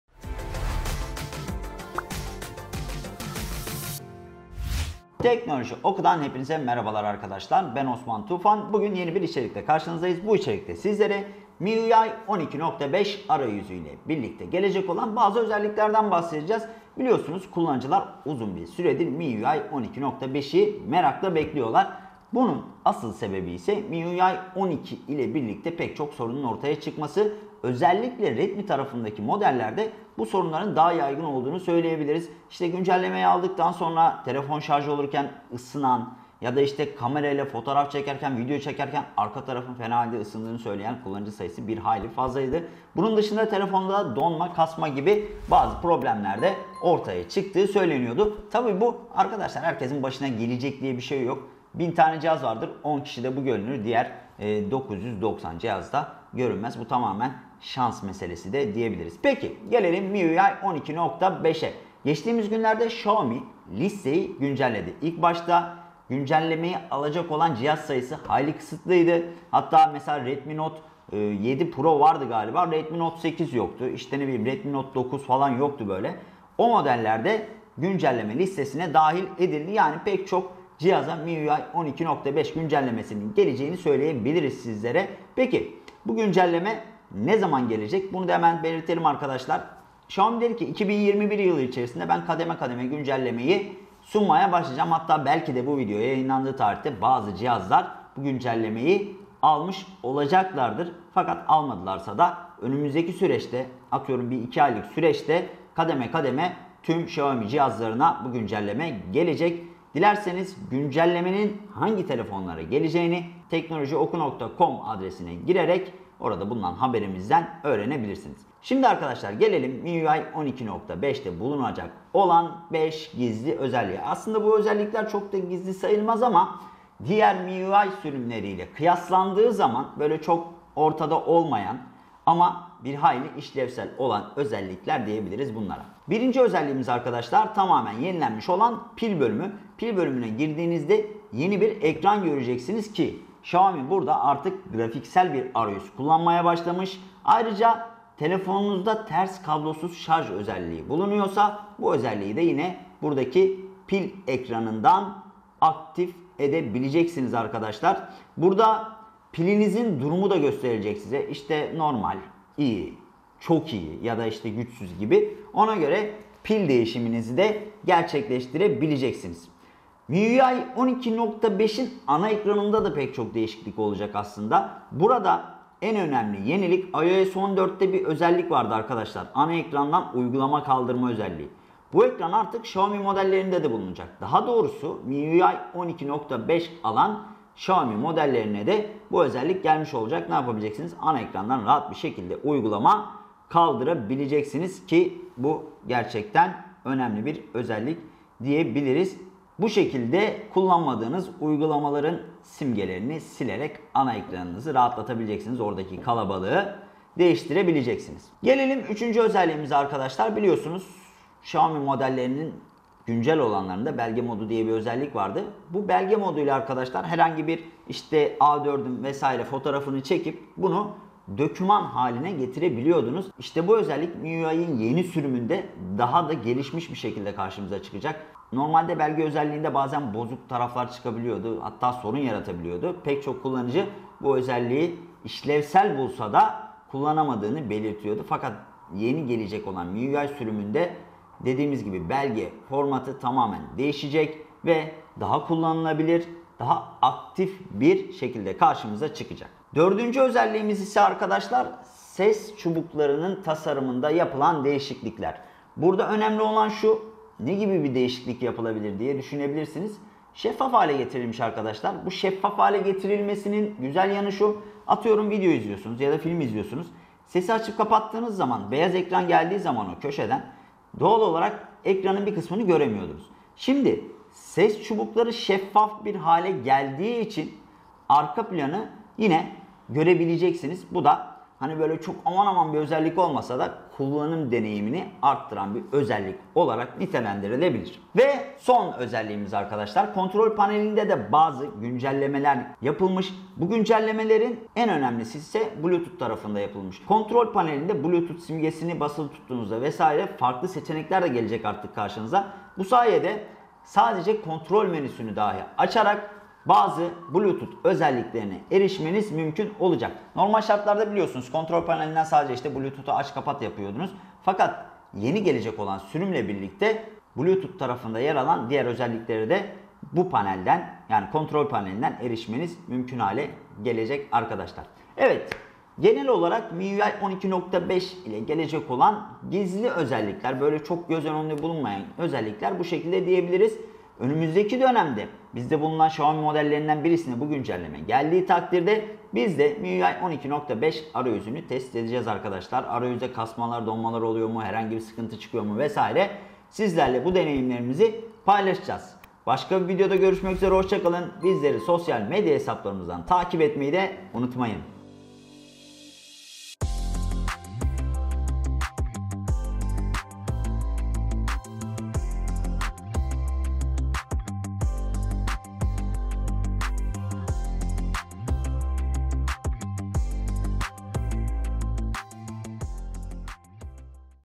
Xiaomi MIUI 12.5 ile gelen 5 gizli özellik!
Akıllı telefon pazarındaki dinamik isimlerden biri olan Xiaomi yeni arayüzü MIUI 12.5 için tüm hazırlıklarını tamamladı. Peki MIUI 12.5 kullanıcılara ne gibi özellikler sunacak?

Bilindiği üzere MIUI çok da stabil çalışan bir arayüz değil. Xiaomi bu arayüzü adam etmek için çok emek sarf etse de her güncellemede farklı modellerde, farklı hatalar ortaya çıkıyor.

Elbette Çin merkezli şirketin tüm hataların üstesinden gelmesi çok da kolay olmuyor. MIUI 12.5 ise herşeyden önce hatasız bir şekilde kullanıcılara sunulmak isteniyor. En azından temel prensip bu.

Xiaomi MIUI 12.5 yükleyecekler! Bunları unutmayın!

Şimdi gelelim MIUI 12.5 ile gelecek 5 gizli özelliğe. Eğer MIUI 12.5'e geçmeyi planlıyorsanız mutlaka bu özelliklere de göz atmanızı tavsiye ederiz.